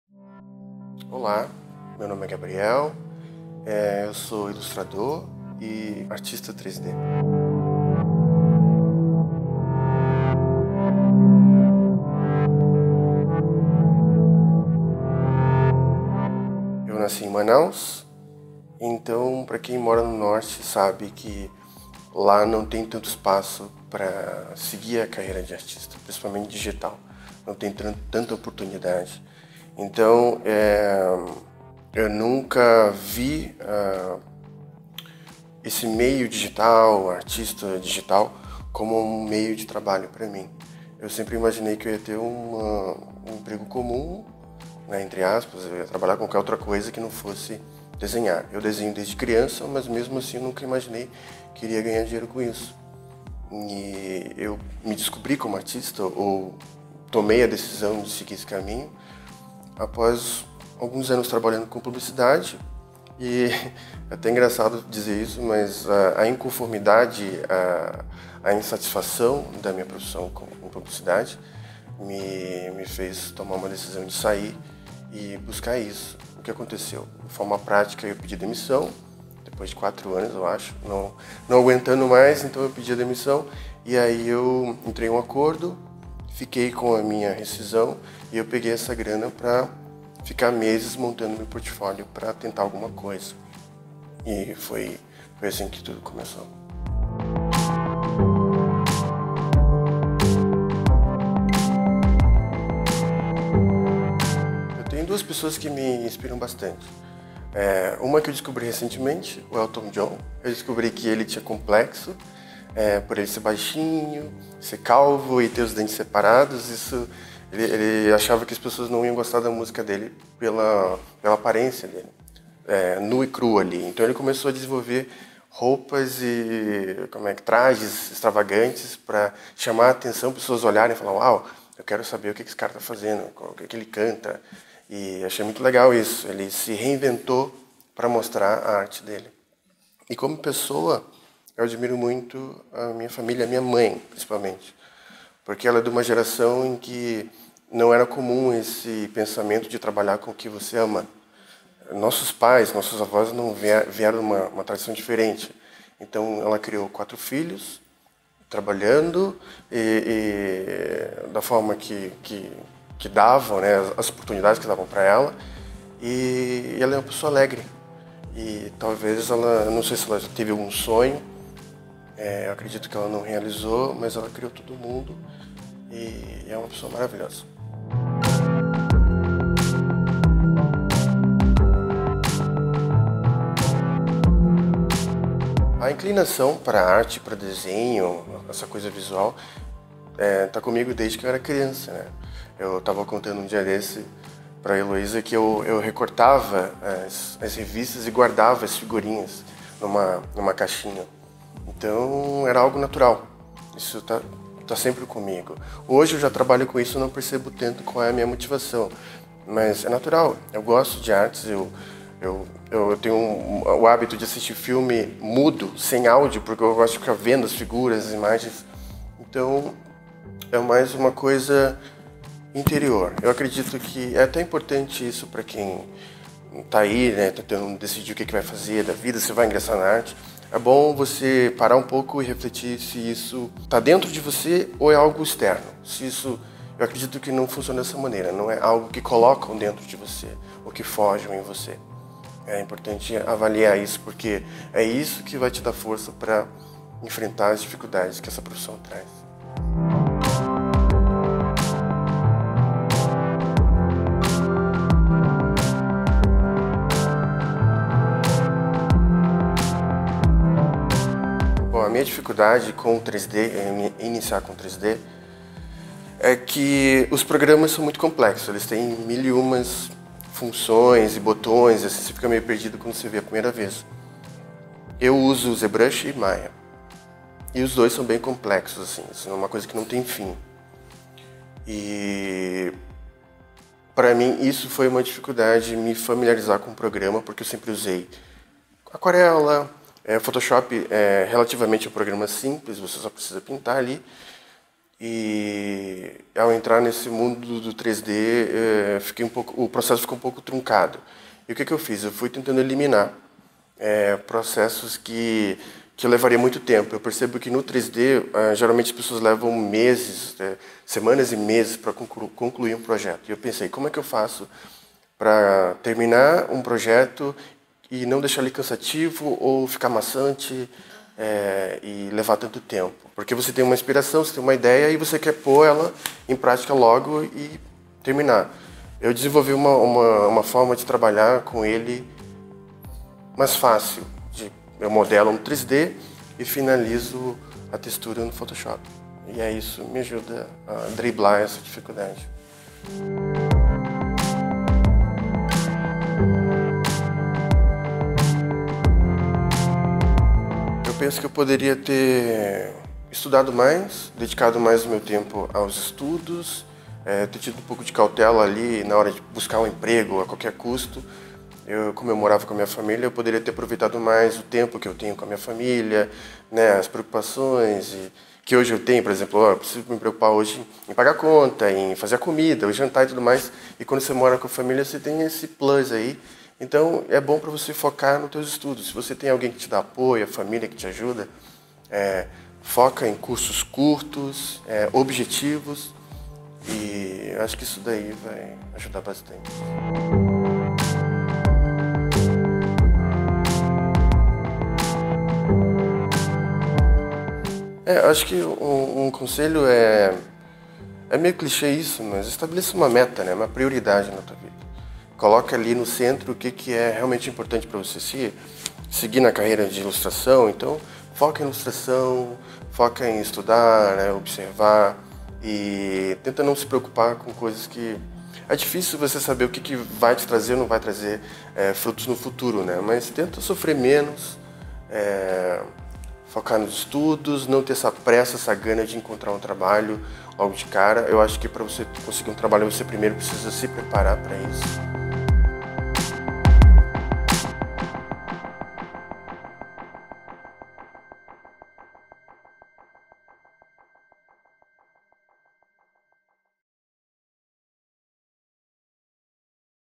Topia Entrevista #28: Gabriel Soares | Topia 2019
O Topia 2019 passou, mas para matar a saudades enquanto esperamos o Topia 3º edição, trazemos uma série de entrevistas com os palestrantes desse ano. Confira sobre as motivações, influências, criações e inspirações desses artistas incríveis! No vídeo de hoje, conversamos com Gabriel Soares!

Entrevistas: Julia Eiko
Captação: Ricardo Oliveira
Edição: Victor Spadotto